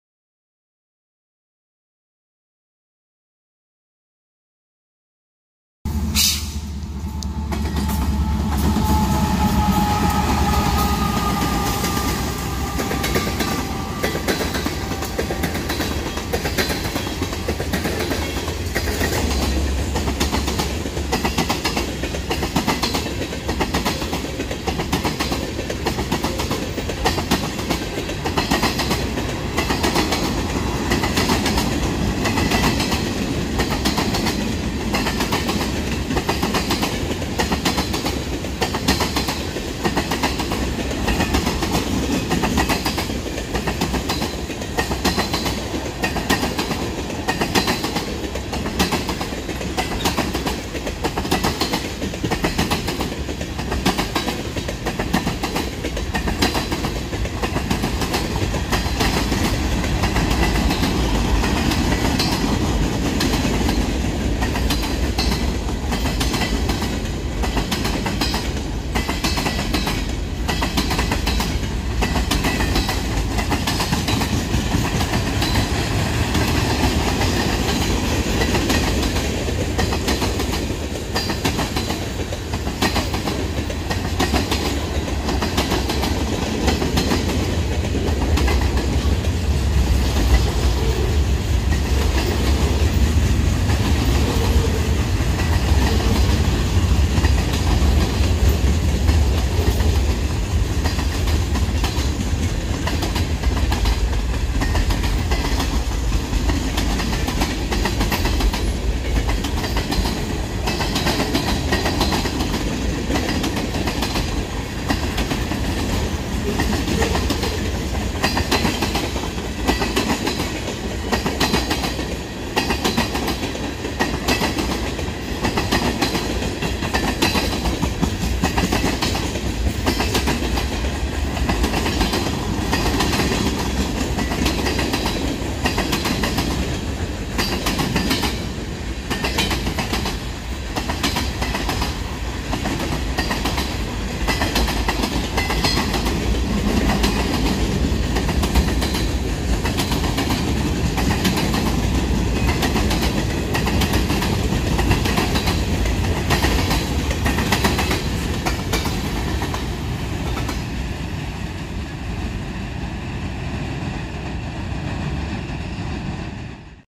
BNSF 7421 WEST AT ROCHELLE,IL
The 7421 is west bound with a manifest as it passes through the Rochelle railroad park.